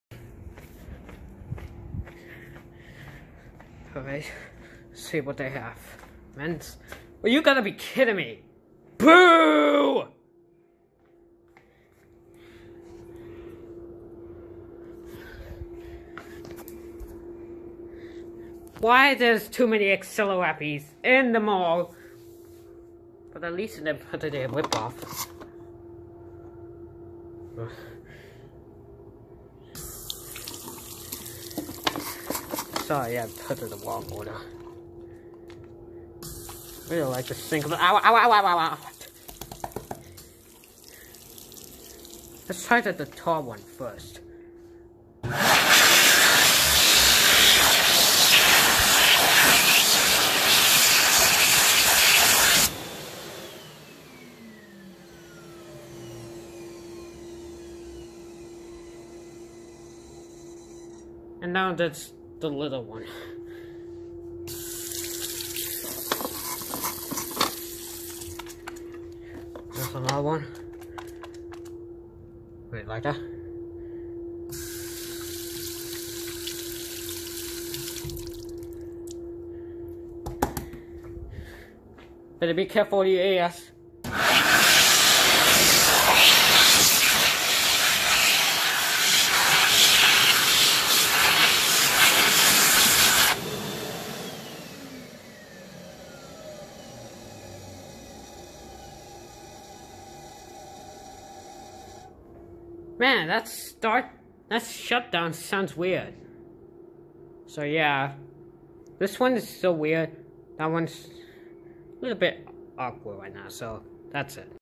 Xlerator Excel Dryer Hand Dryer At Manassas Mall in Manassas VA
WHY THERE ARE NO DYSONS IN THEIR
What A Xlerappy
[Model-XL-SBX]